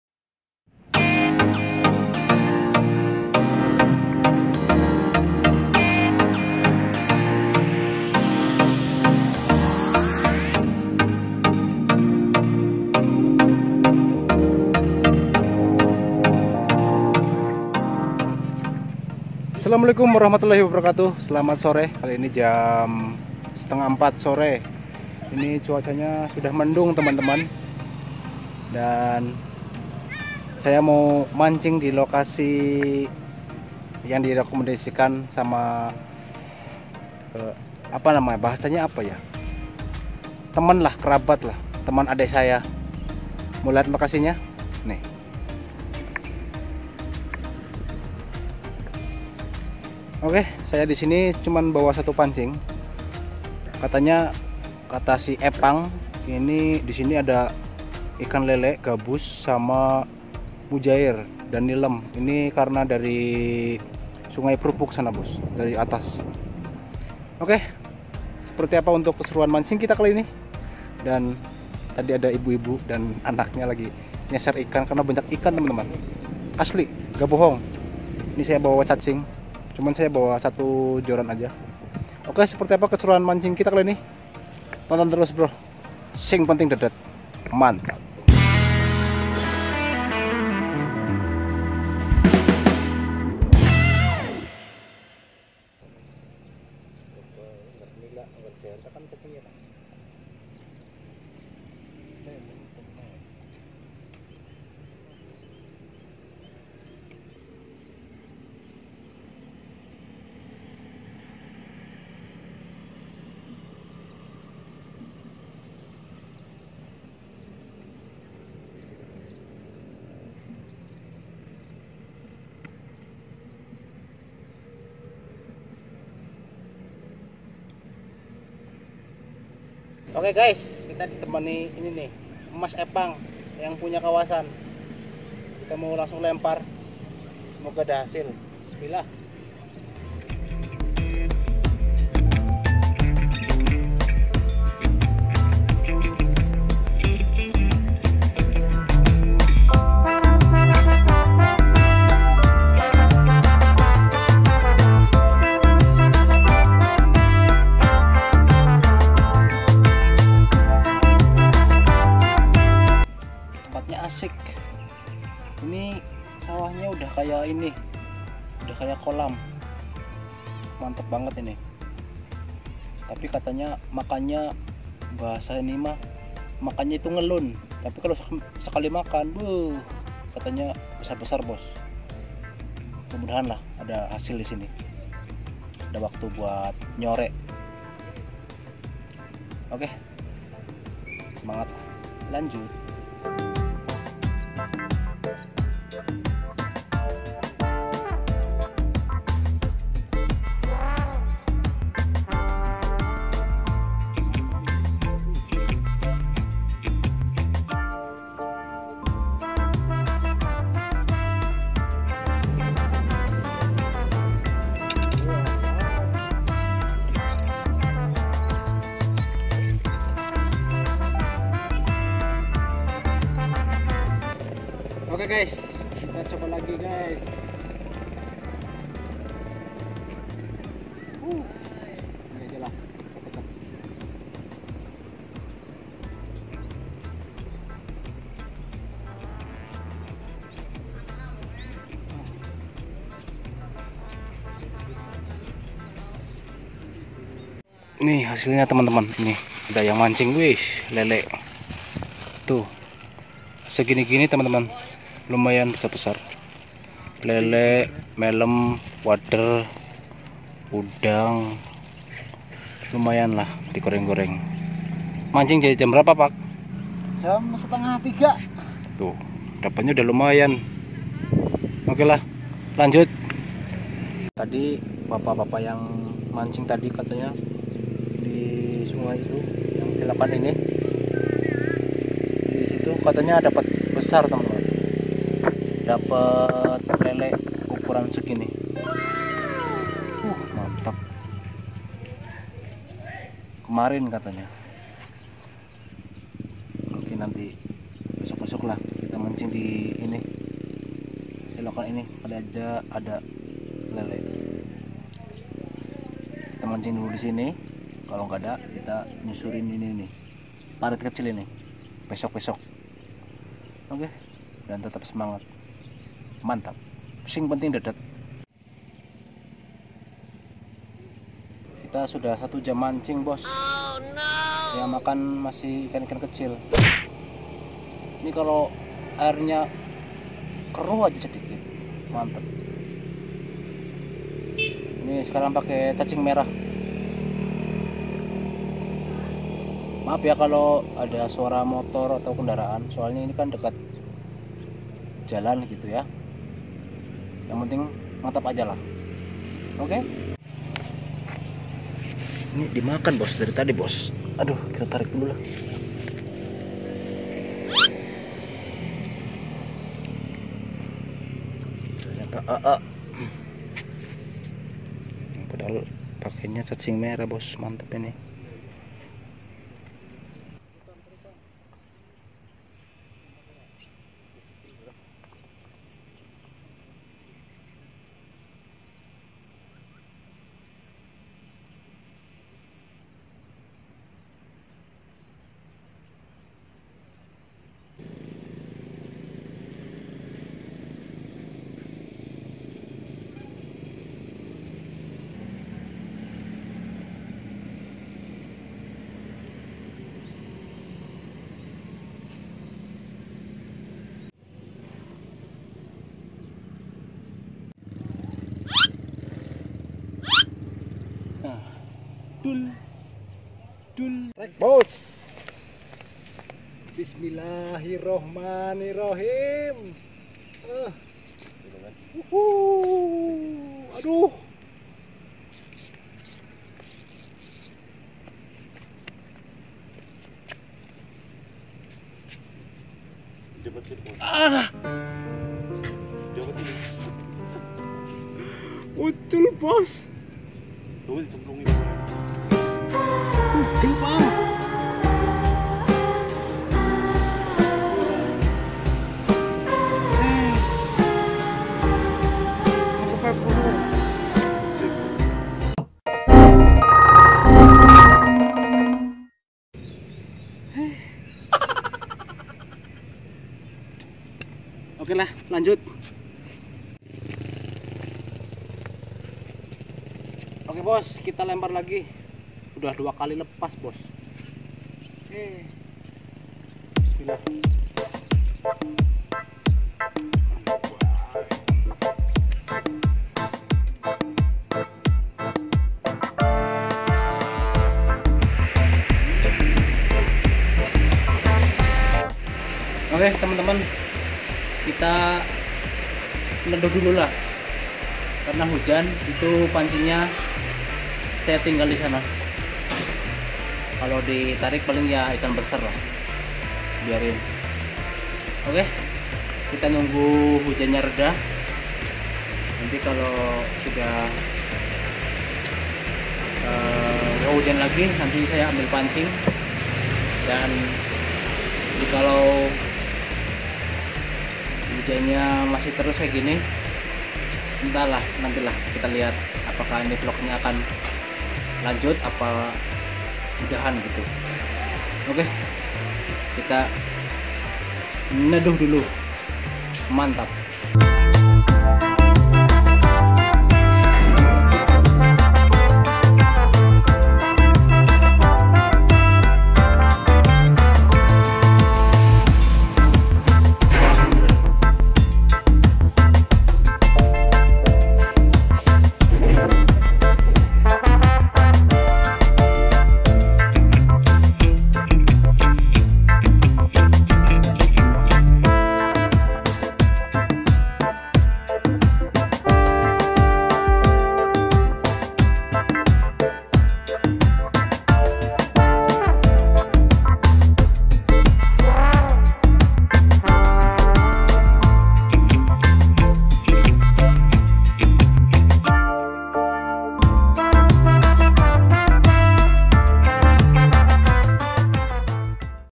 Persawahan Rasa Empang - Purbayasa Tonjong
Spot persawahan desa purbayasa tonjong brebes
Ikan berasal dari sungai prupuk
-Nila
-Lele
-Gabus
-Udang
-Nilem
-Wader
Dll

Persawahan Rasa Kolam Ikan, REEL MENJERIT SAAT DI TARIK IKAN BESAR SAMPAI PUTUS